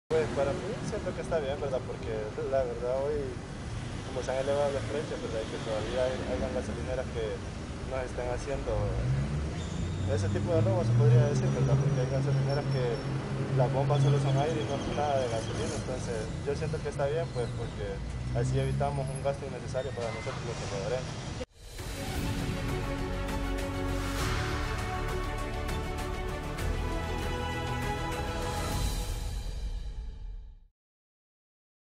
Conductor agradece la inspección que realiza Defensoría del Consumidor en estaciones de combustible
11MedidasAnteLaInflación | Omar Ávila respaldó las acciones del Gobierno del Presidente Nayib Bukele, como las inspecciones en el precio de la gasolina. Considera que esto beneficia a los salvadoreños.